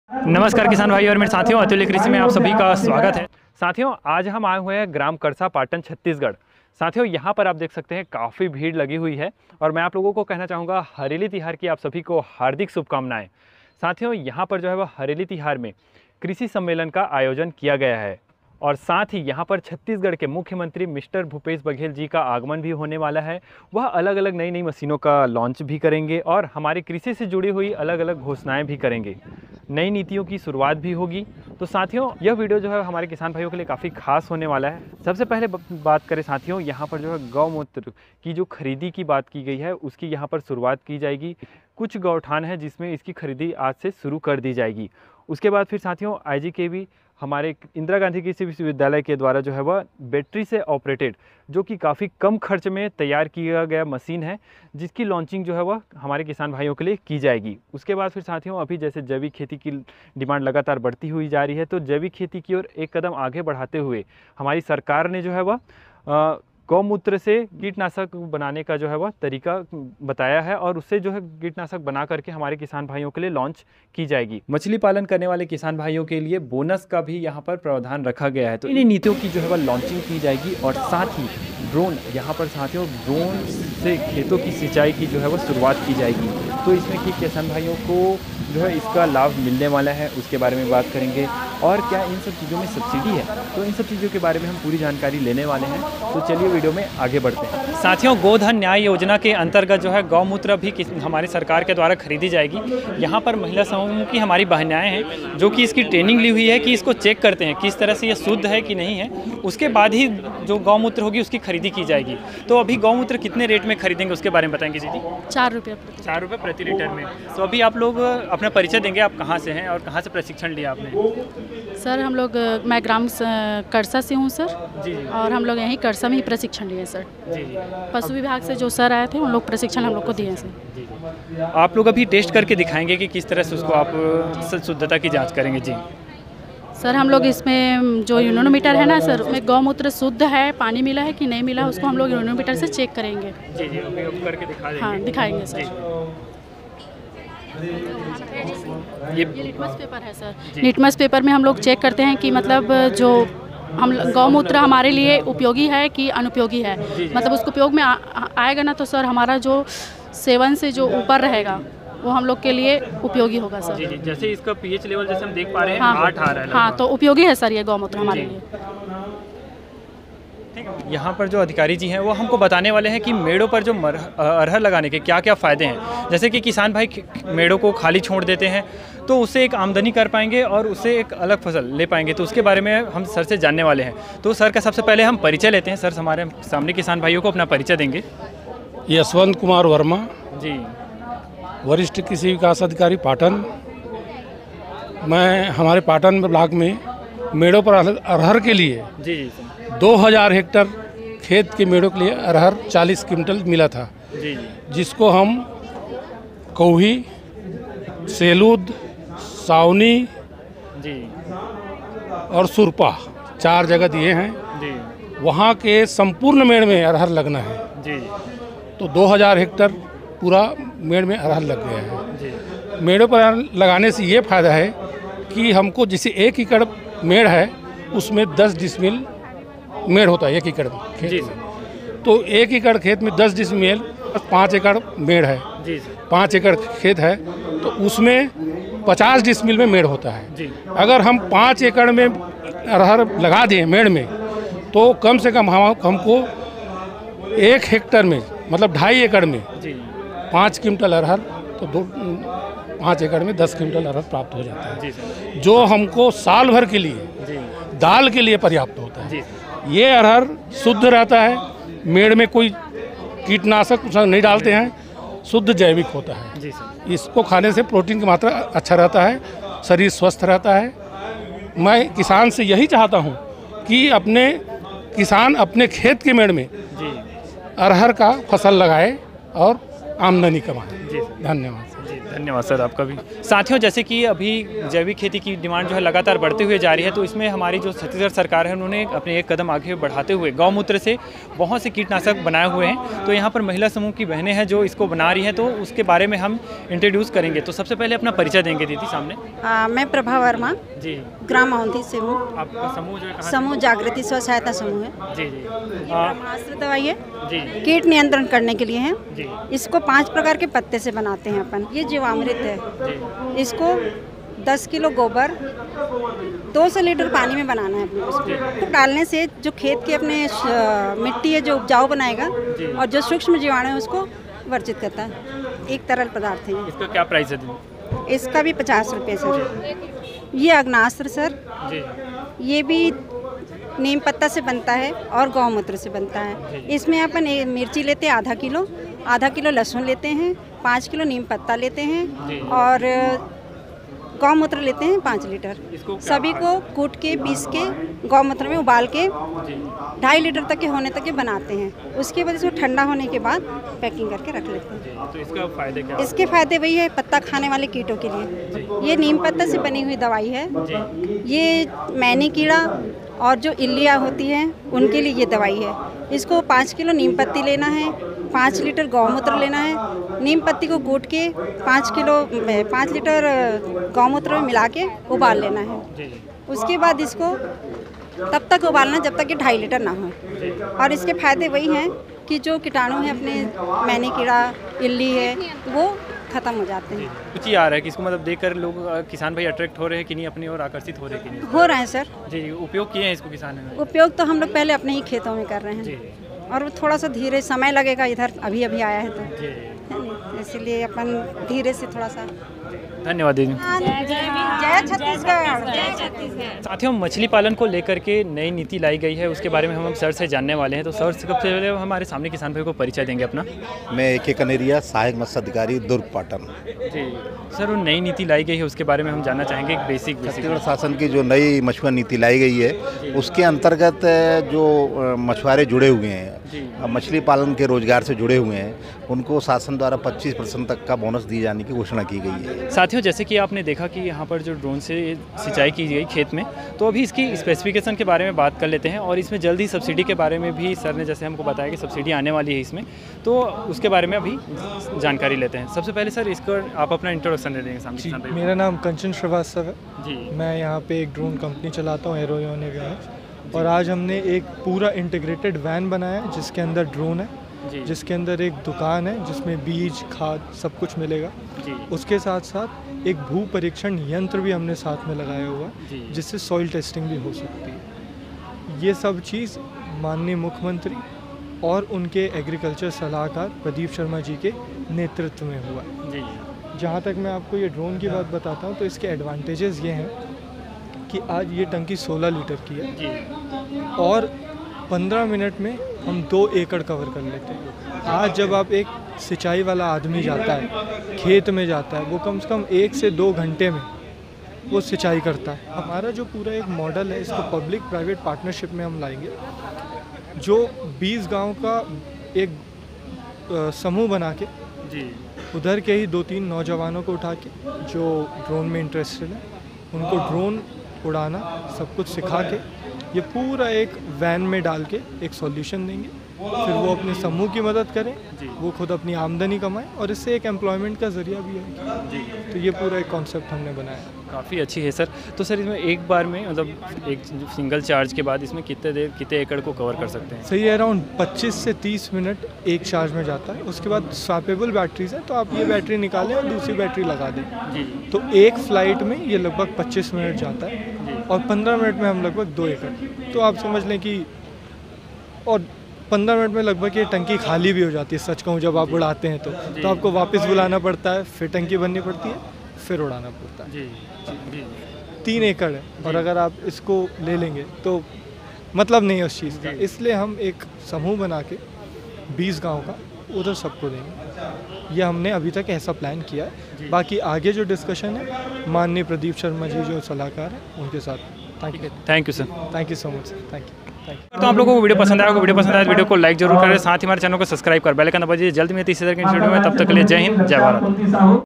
हरेली तिहार में छतीसगढ़ किसान भाईयों के लिए नई कृषि तकनीक और नीति की घोषणा । Hareli Tihar Special ।
हरेली तिहार में किसान सम्मेलन 2022 का आयोजन किया गया है।
जिसमें छत्तीसगढ़ के मुख्यमंत्री मिस्टर भूपेश बघेल जी कृषि कार्यों व नई तकनीकों मैं घोषणा करने वाले हैं।
गोमूत्र की खरीदी
छत्तीसगढ़ में ड्रोन से खेतों की सिंचाई
Igkv launch लो कॉस्ट बैटरी ऑपरेटेड मशीन।